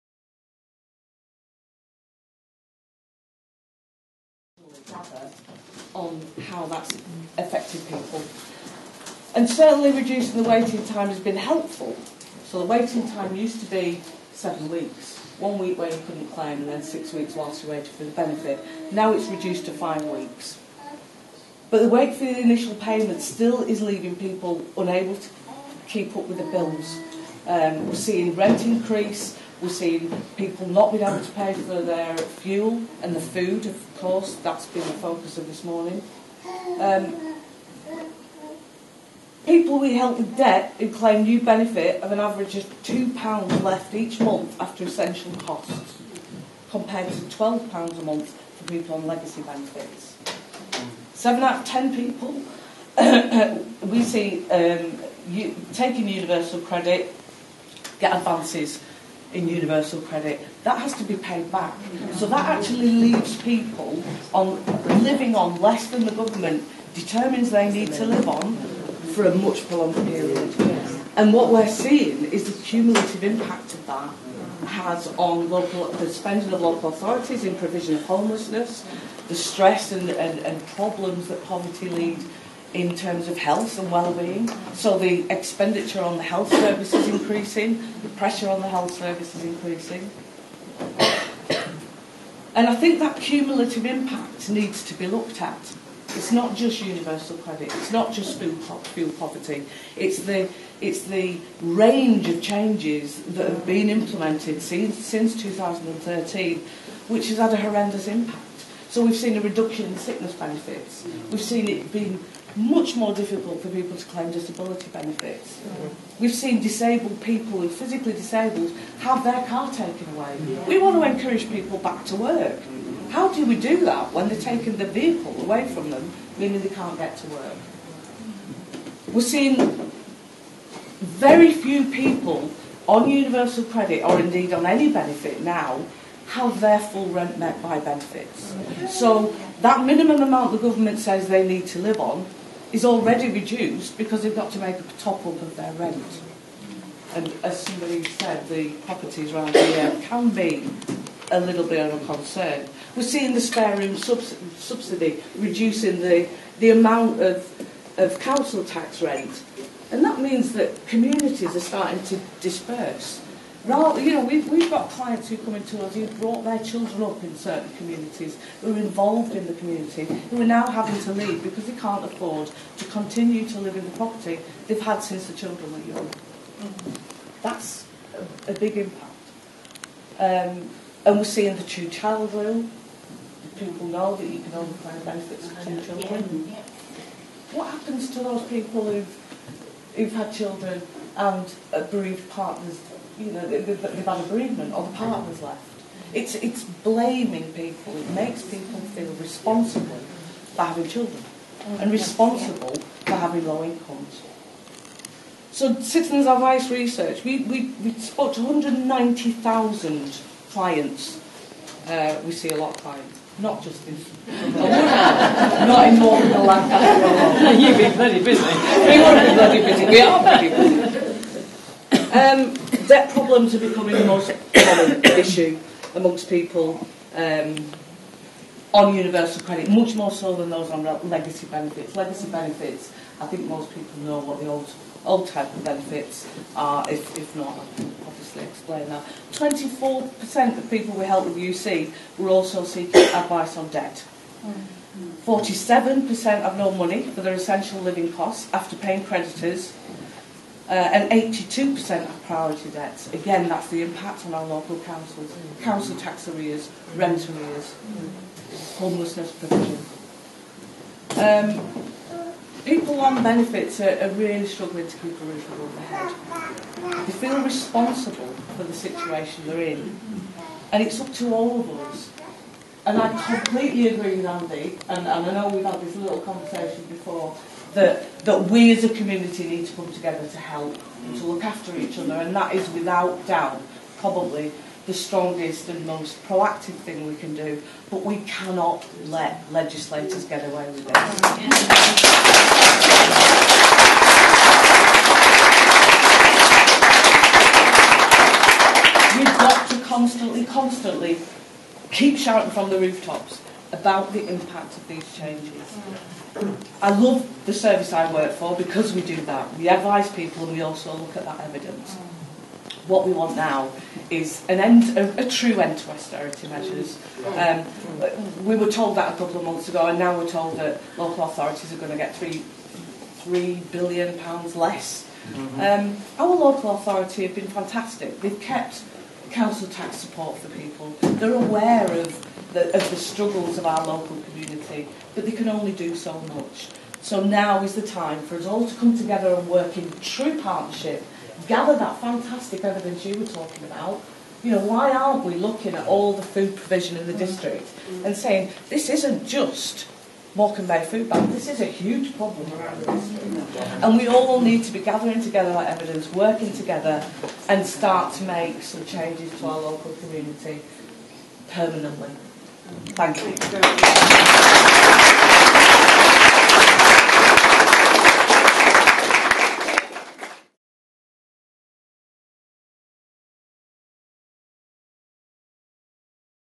MORECAMBE BAY FOODBANK 'Benefits' with Helen Greatorex from Citizens Advice
Benefits with Helen Greatorex from Citizens Advice. Morecambe Bay Foodbank welcomed the MPs, Heidi Allen & Frank Field on their latest stop on a fact finding Anti-Austerity Tour of the UK. The pair sit on the Work and Pensions Select Committee. They heard some powerful and passionate local testaments and talked to real people who experience real life poverty.